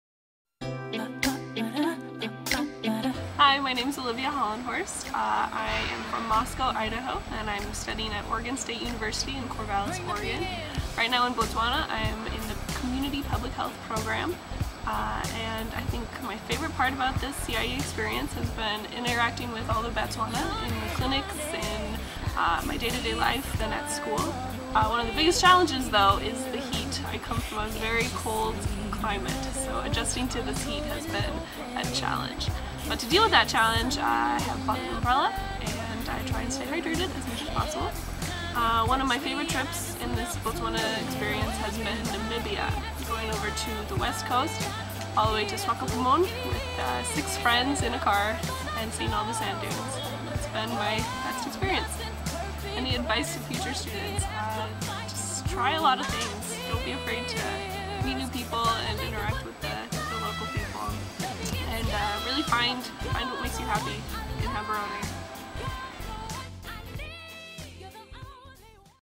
CIEE Gaborone Student profile: Olivia Hollenhorst
Get to know one of our students from the CIEE spring 2012 semester!